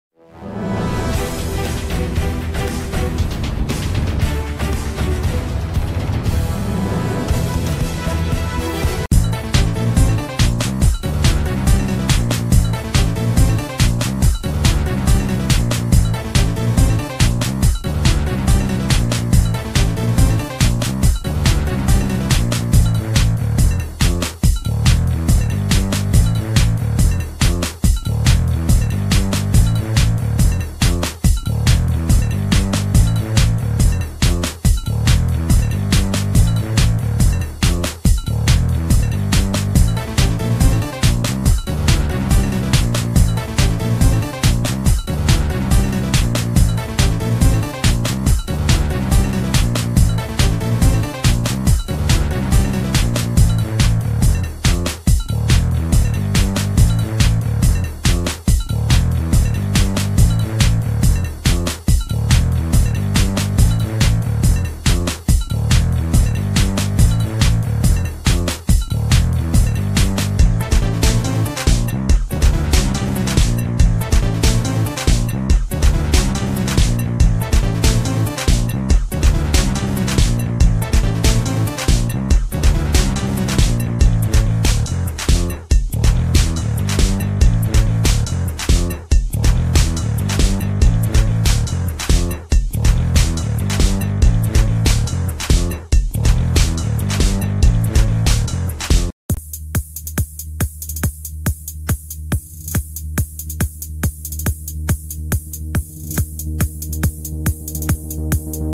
Neymar: PSG owner makes huge transfer statement amid Man Utd and Real Madrid links
Many Thanks./